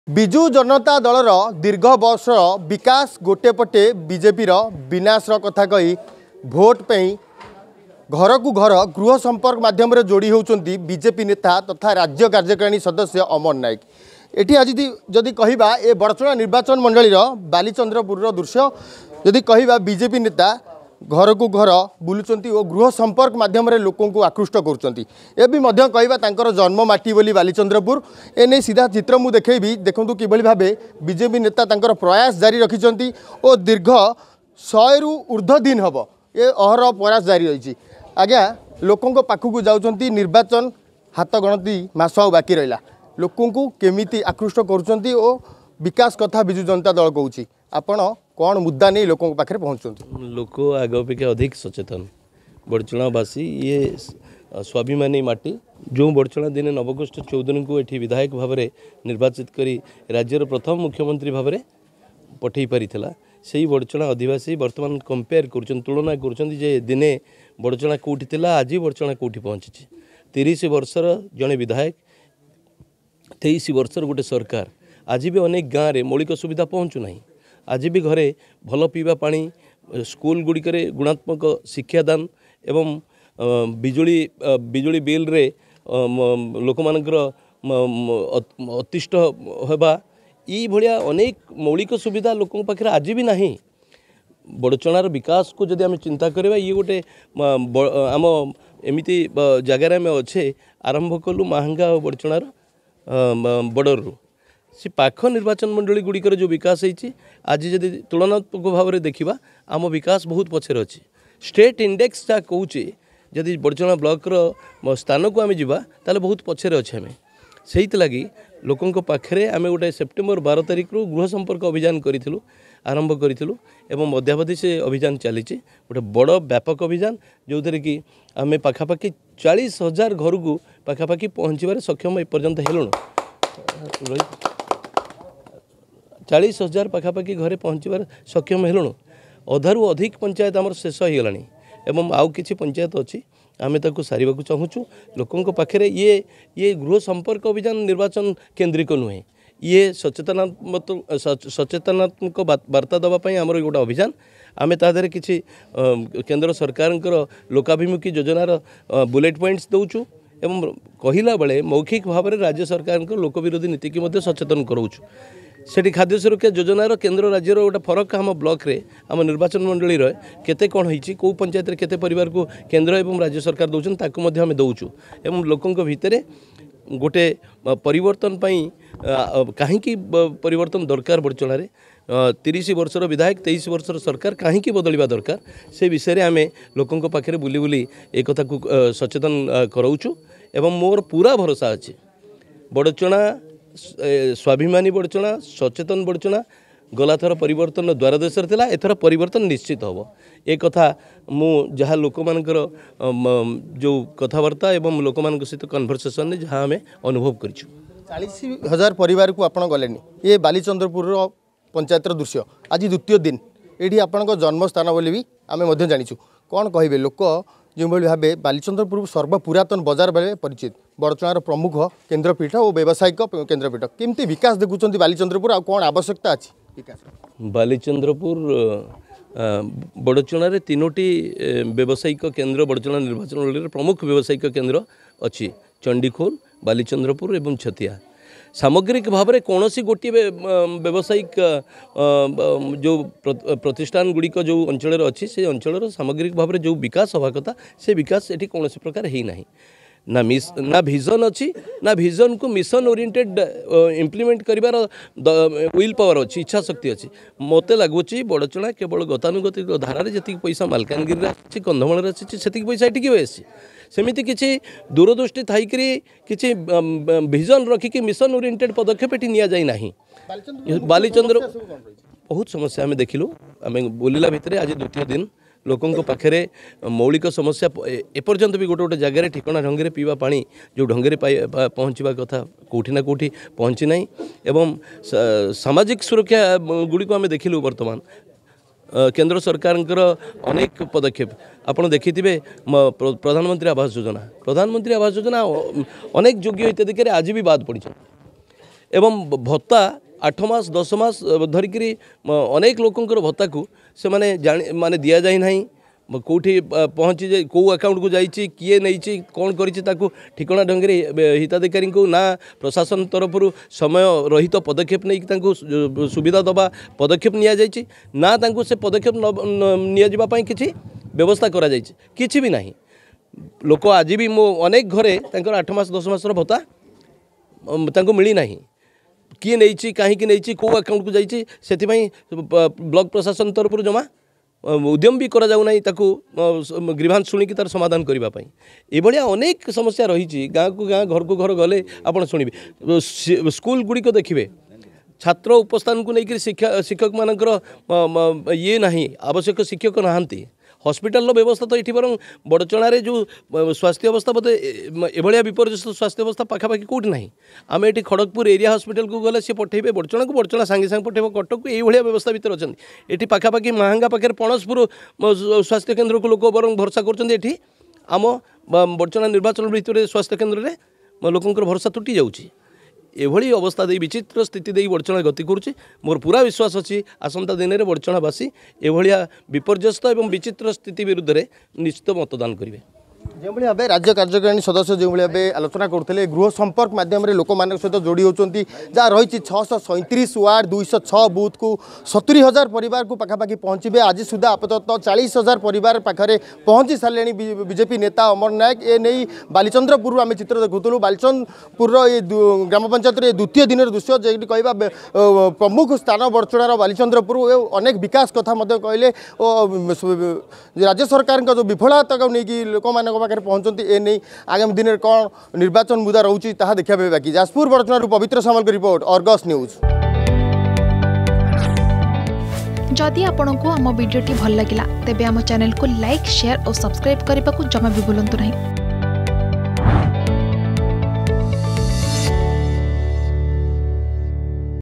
ଗୃହ ସମ୍ପର୍କ ଅଭିଯାନ ମାଧ୍ୟମରେ ଘରକୁ ଘର ବୁଲୁଛନ୍ତି ବଡ଼ଚଣା ବିଜେପି ନେତା || BJP Gruha samparka
ଗୃହ ସମ୍ପର୍କ ଅଭିଯାନ ମାଧ୍ୟମରେ ଘରକୁ ଘର ବୁଲୁଛନ୍ତି ବଡ଼ଚଣା ବିଜେପି ନେତା ।